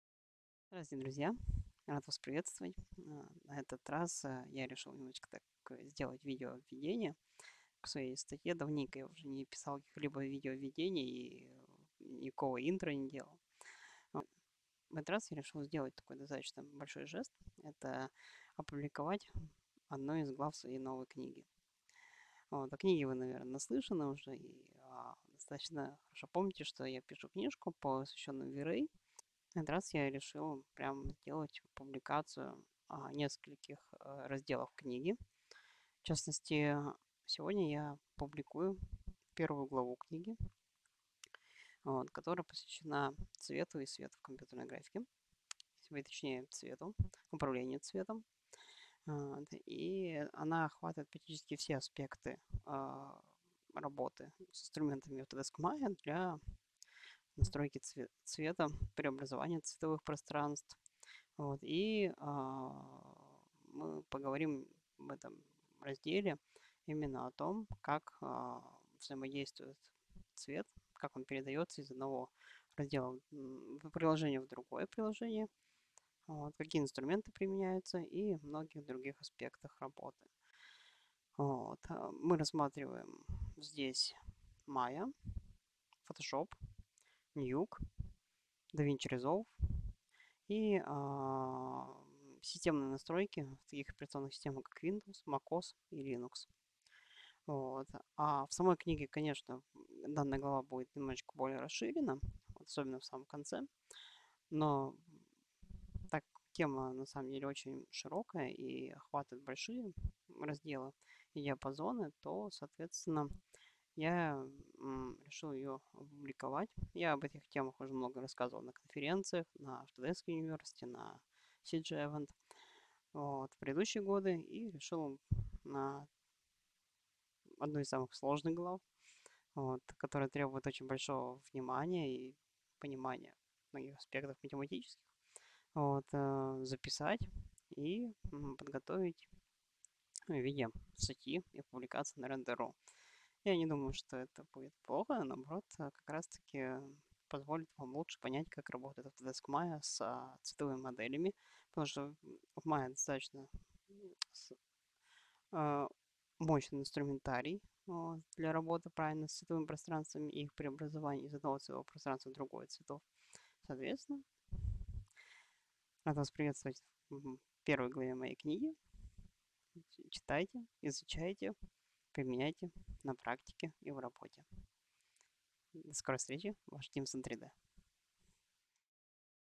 Первая глава моей новой книги
Коллеги и друзья, рад представить вам первую главу моей новой книги, посвященной визуализации в Autodesk Maya и системе визуализации V-Ray.
Глава 1 "Свет и цвет в компьютерной графике" посвящена основам работы компьютеров и программ с цветом и реализацией цветовых моделей в различных приложениях и операционных системах.